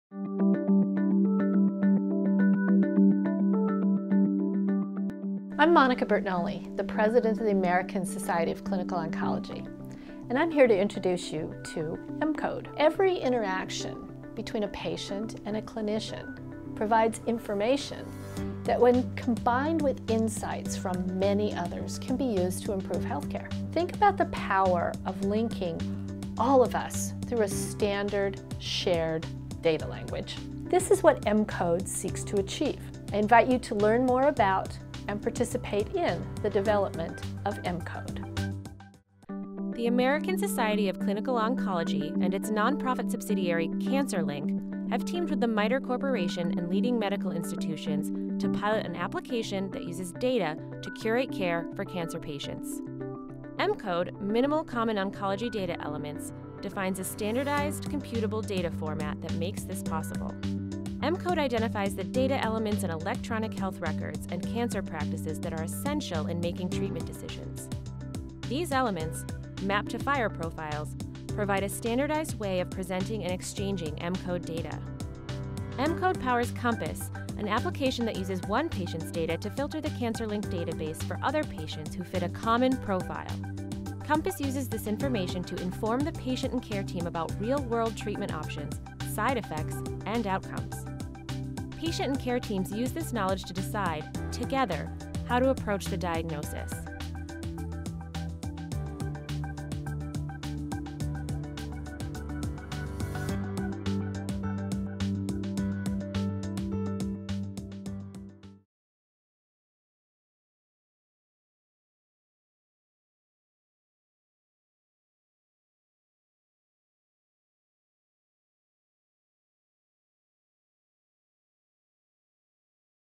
mCODE and Compass
MITRE is collaborating with ASCO, CancerLinQ, and other clinical experts to develop mCODE (Minimal Common Oncology Data Elements). mCODE models a standard set of interoperable data that will be collected for every cancer patient, with the objective of substantially improving data for cancer care and research. mCODE is a new data standard under development that will facilitate insight into high-quality data for multiple cancer use cases.  MITRE is also developing and piloting Compass™, a first-in-class, open-source application to collect mCODE, from electronic health records (EHR). Compass is a SMART (substitutable medical applications and reusable technologies) on FHIR (fast healthcare interoperability resources) application that extracts mCODE data elements from electronic health records to deliver reports to providers and patients - empowering better diagnosis, improved care planning, and shared decision making at the point of care.